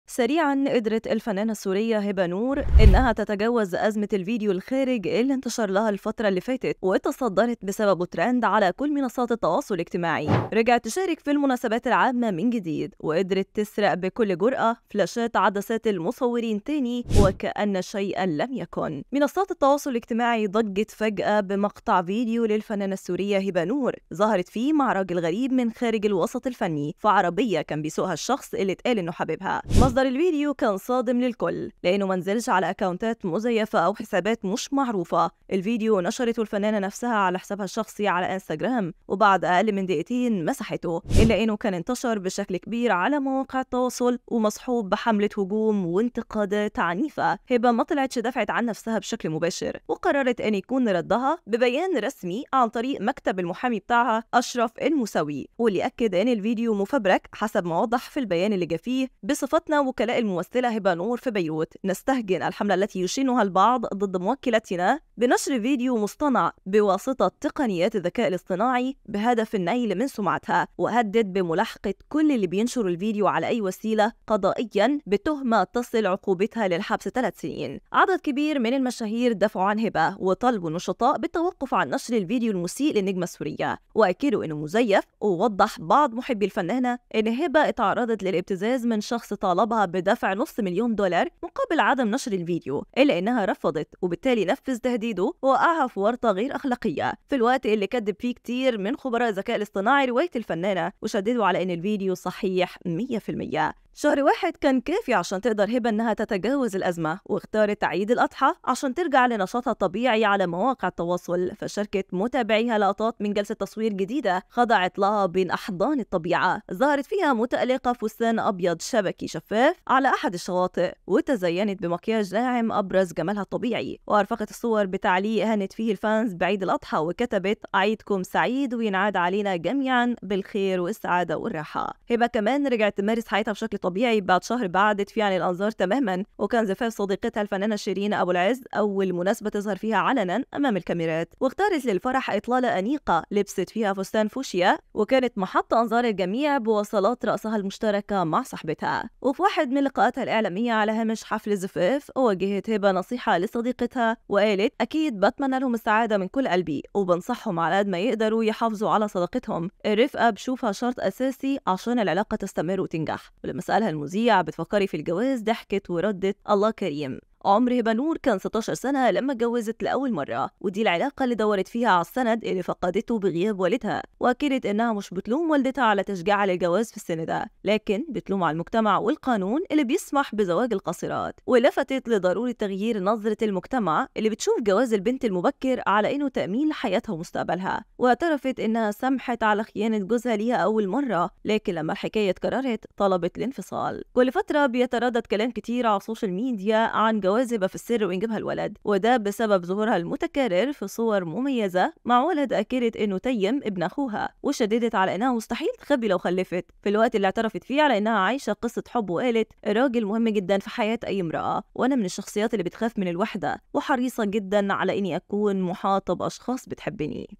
هبة نور تتجاوز أزمة الفيديو الخارج وترقص في زفاف صديقتها وتكشف موقفها من الزواج
تابعنا على منصة كل النجوم
سريعاً قدرت الفنانة السورية هبة نور تجاوز أزمة الفيديو الخارج اللي انتشر لها الفترة ايللي فاتت واتصدرت بسببه الترند على كل منصات السوشيال ميديا، رجعت تشارك في المناسبات العامة من جديد وقدرت تسرق بكل جرأة فلاشات عدسات المصورين من تاني وكأن شيئاً لم يكن.